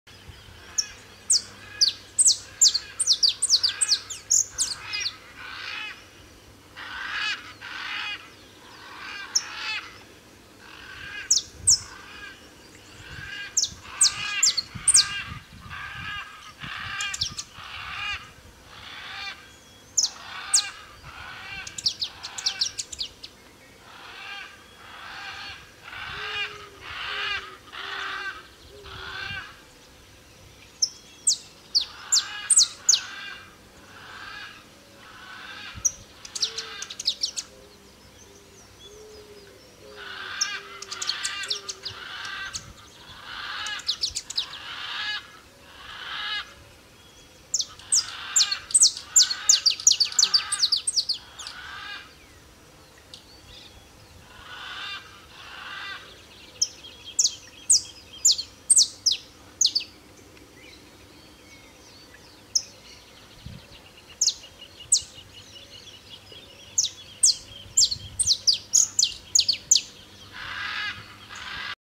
O canto do canário-da-terra verdadeiro livre na natureza (sicalis flaveola)
O  canto do canário-da-terra macho verdadeira cantando livre na natureza. Tupi Paulista São Paulo.